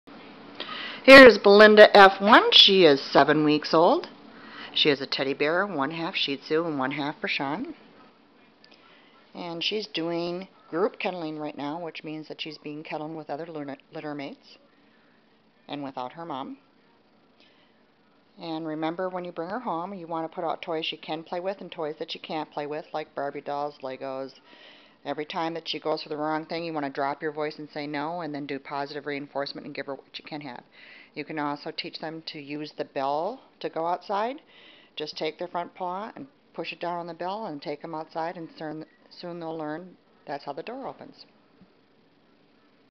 belinda03172018f1 on 5 6 2018
1/2 shih tzu & 1/2 bichon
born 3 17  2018
belinda f1 are  available $575
x are SOLD
x are available $725
x  are SOLD
m=male f=female
mother 10.6# father 13#
range 9-15#
7 weeks on 5/5
8 weeks on 5/12
9 weeks pickup 5/19
non-shed hypo-allergic
7   weeks